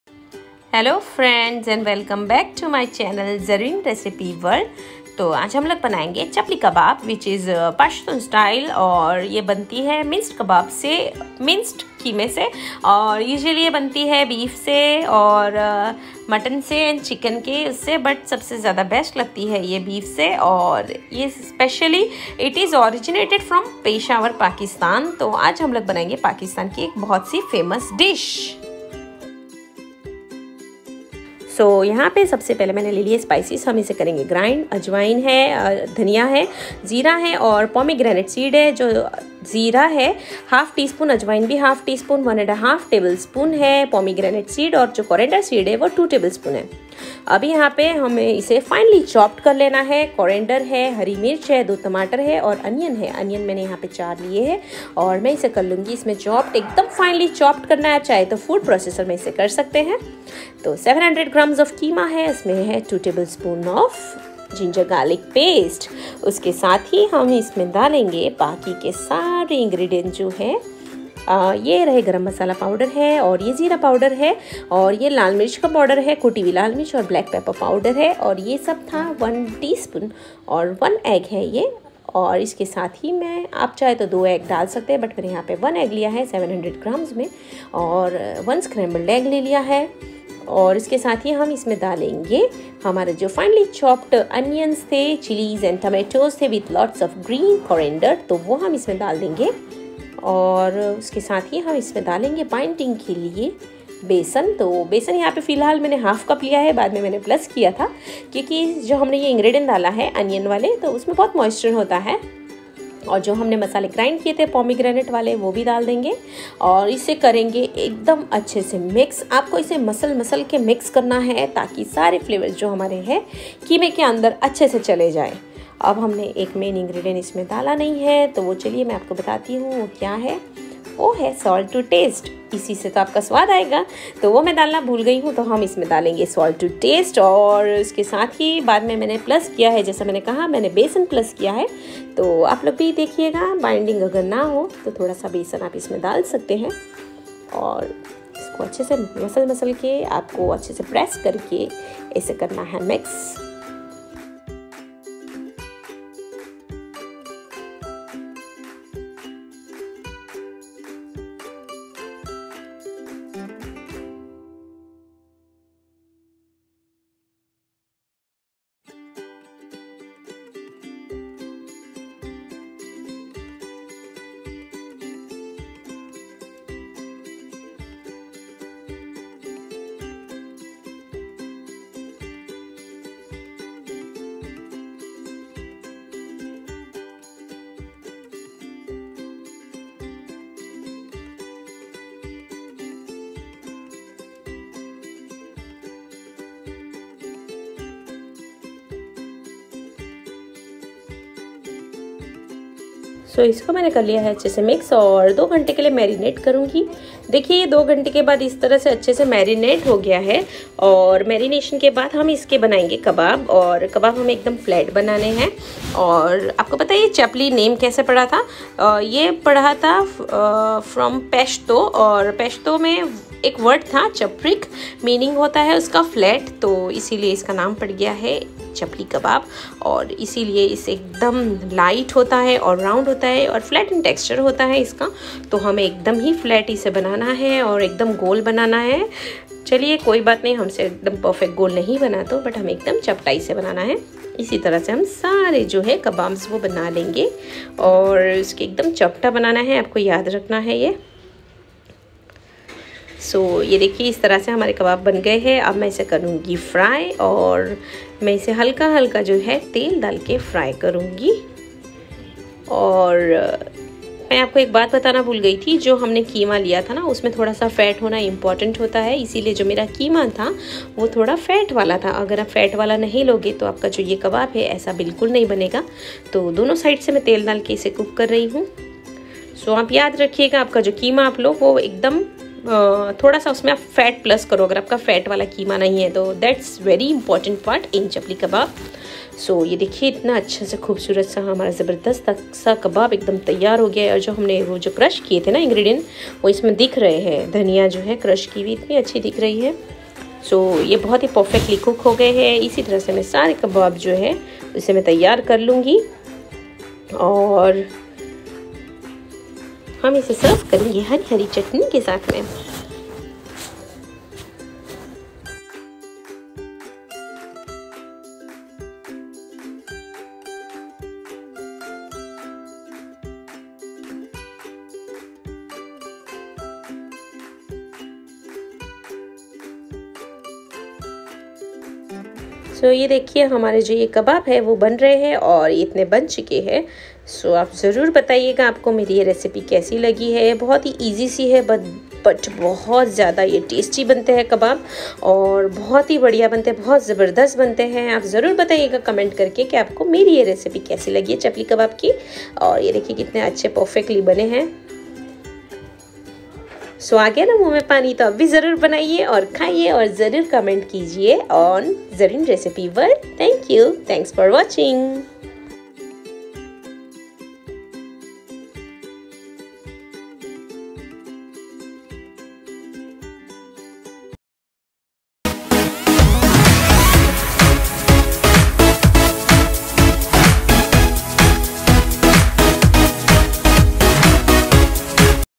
Chapli Kebab/chaplikebab /pashto / Street food/ZRN
Basically this chapli kebab is pashto style and in pashto they say chaprikh which means flat and round so that is why the shape of this kebab is flat and round.